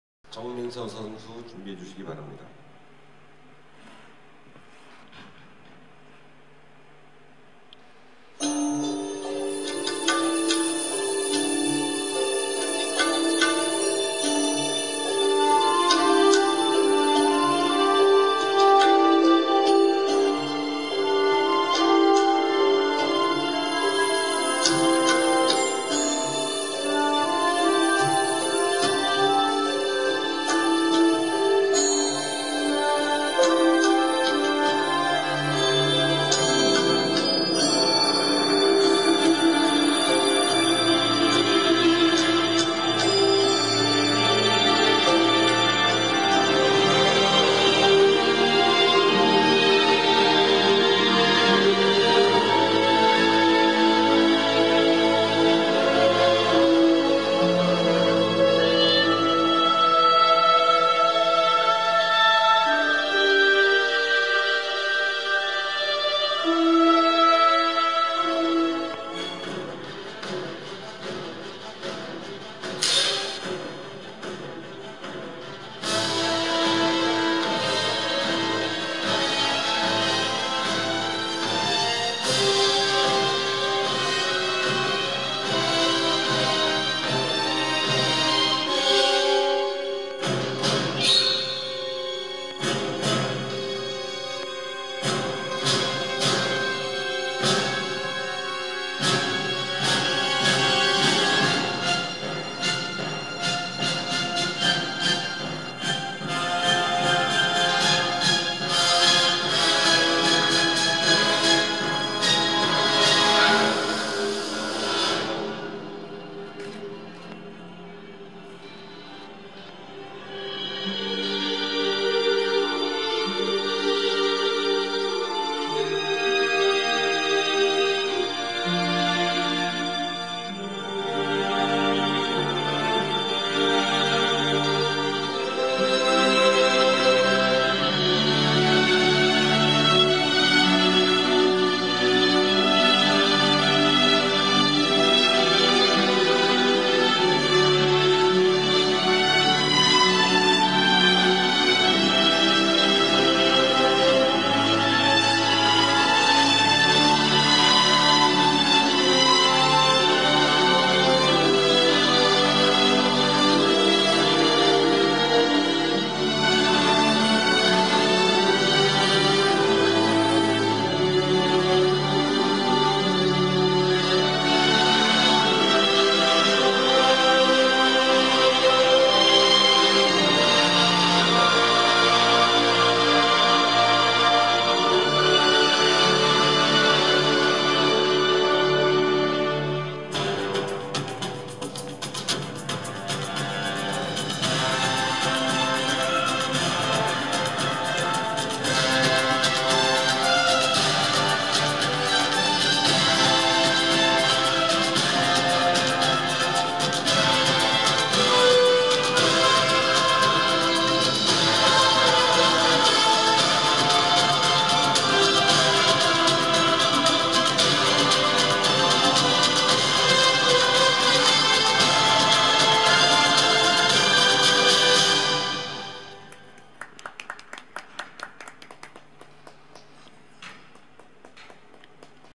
정민서 Min-Seo JEONG | 6급 FS심사 07 | 2017-11-04 4차 5-8급 승급심사 | 해오름초 5
김리현,도윤정,정민지,이주현,김진서,홍세라,고순정,김도연,정민서,박연정,이동혁,조승연,원채은

Figure Skating Level Test | (Nov.04) 2017. Taereung Indoor Ice Rink
피겨스케이팅 3차 5-8급 승급심사 |